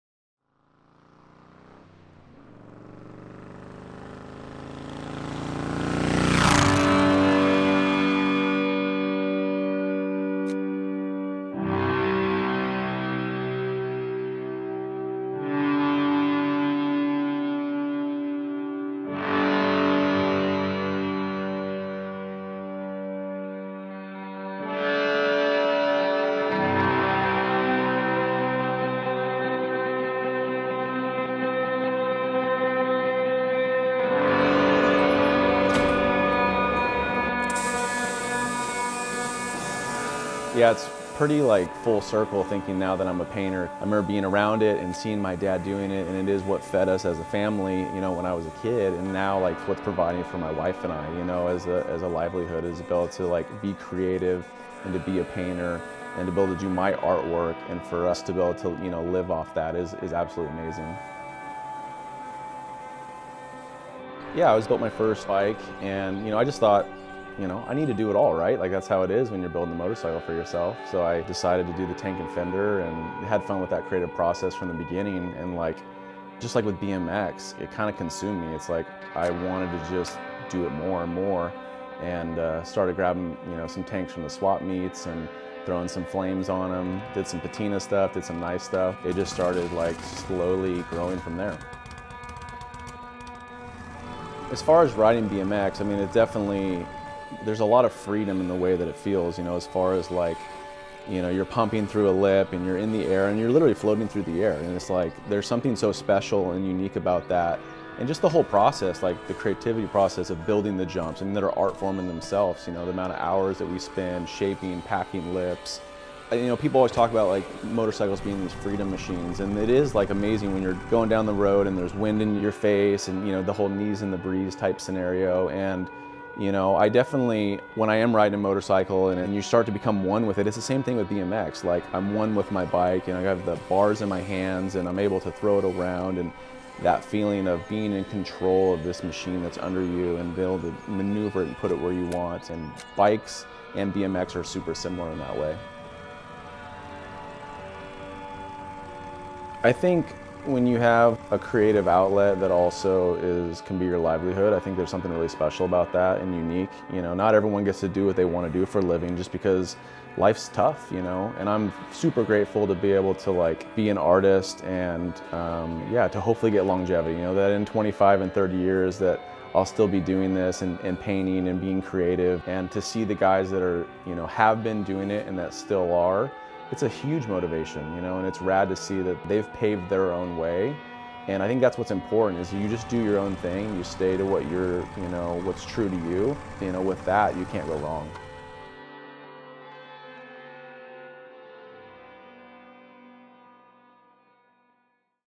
Nathan Sykes - BMX - Paint - Choppers
Nathan is an incredibly talented artist and a bmx legend. This is his story.

Follow below